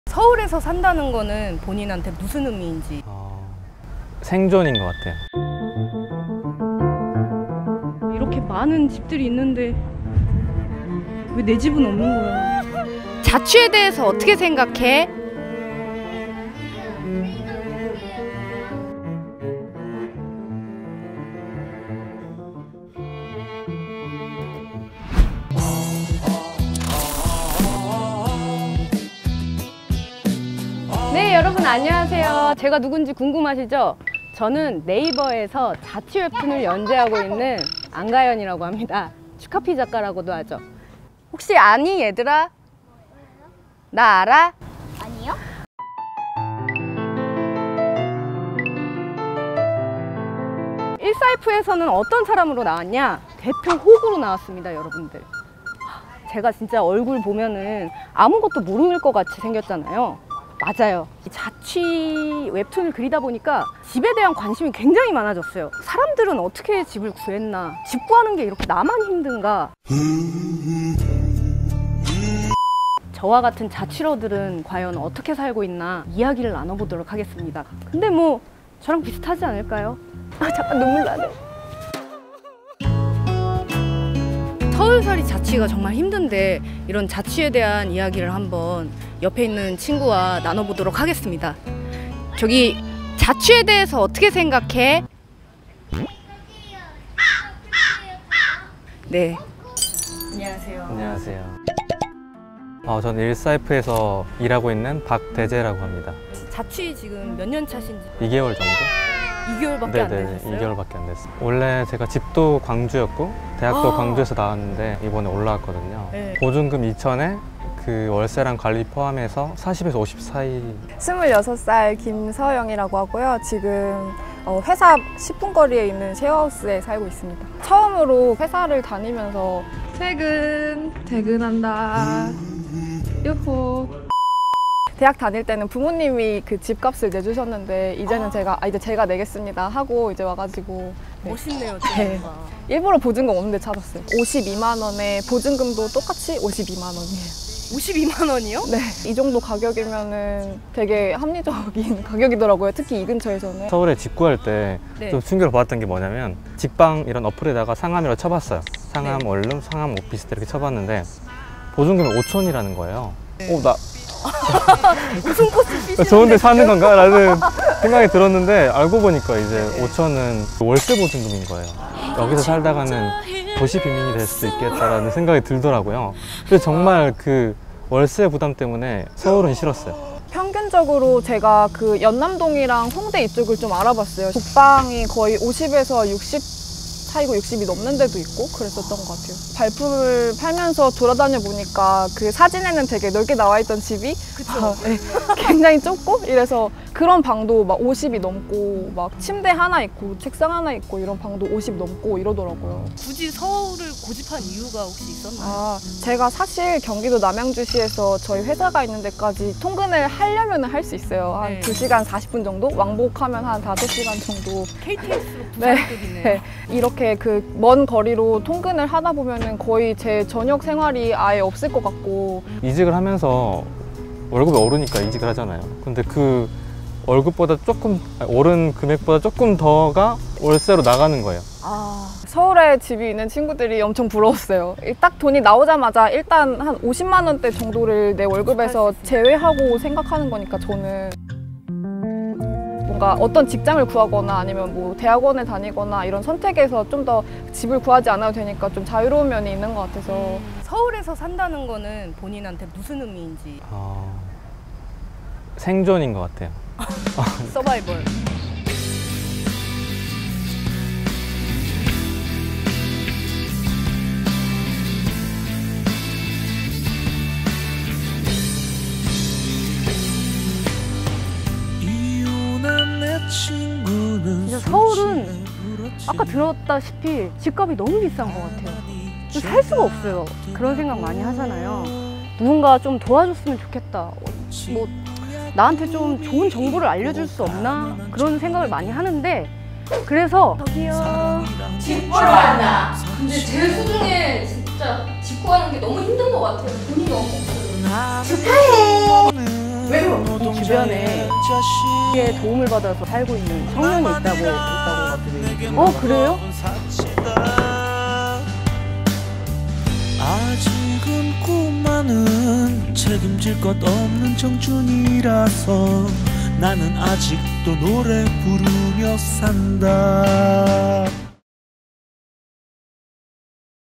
눈물 나는 자취러들의 서울살이
"저한테 서울은 '생존'이에요"
"숨만 쉬어도 월급의 반토막이 월세로 나가요"

대한민국 면적의 0.61%, 서울.
부푼 꿈과 설레는 마음으로 상경하지만...
집을 구하는 첫 관문부터 참 쉽지않다.

이렇게 집이 많은데, 내 몸 하나 누울 곳 찾는 게 왜 이렇게 힘든지...

삭막한 도시에서 나를 도와줄 사람은 정말 없는걸까...?

짠내나는 청년들에게 한줄기 빛이 되어줄 '현자'를 찾기 위해 '자취로운 생활' 작가 츄카피 14F에 왔다! 과연 첫번째 '현자'는 누구일까?